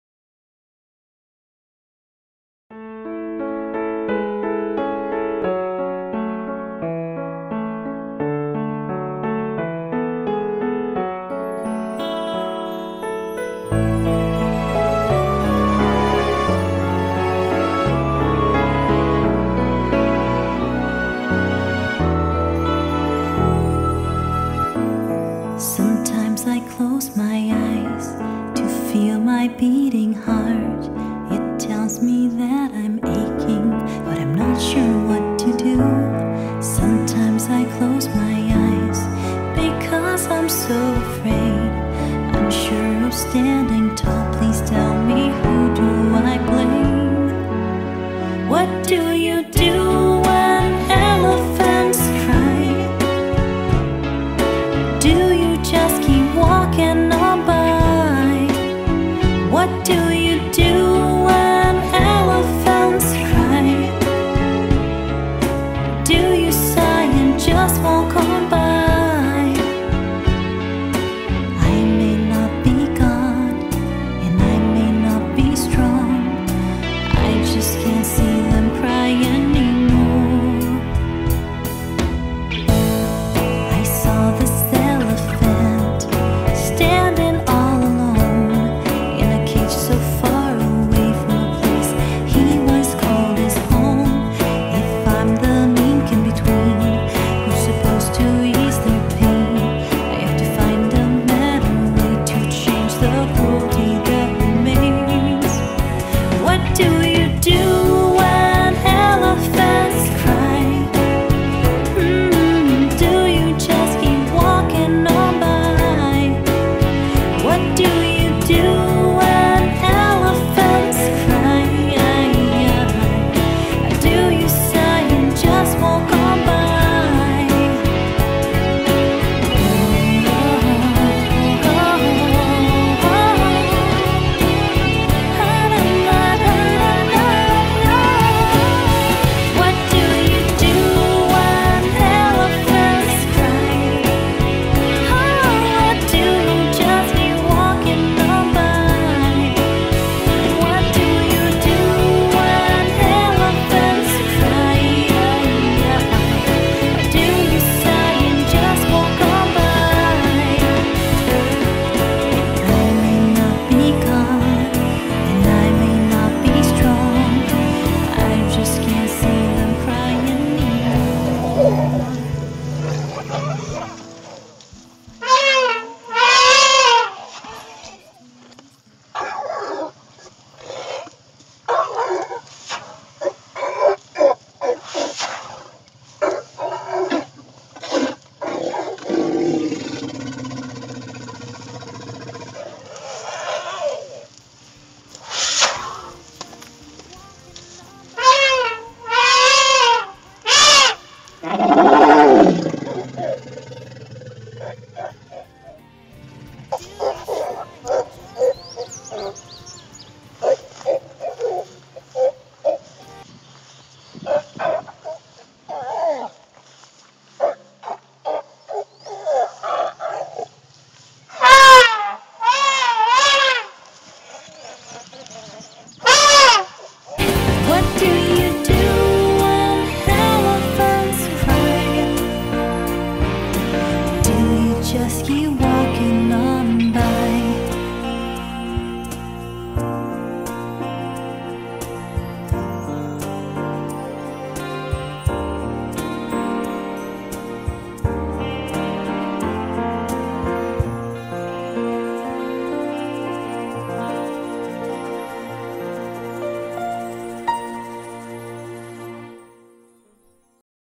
When Elephants Cry
Video by Courtney Scott, about the Woodland Park Zoo elephants and their living conditions.